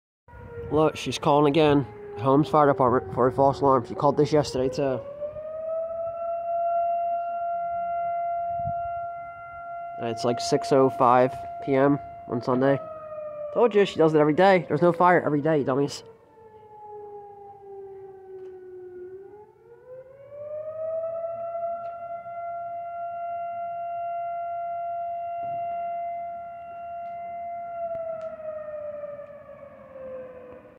my ex-girlfriend just called him his fire department today she called it yesterday nine companies
my ex-girlfriend just called homes fire department today and she just called it yesterday too as well and she called nine fire departments yesterday for all false alarms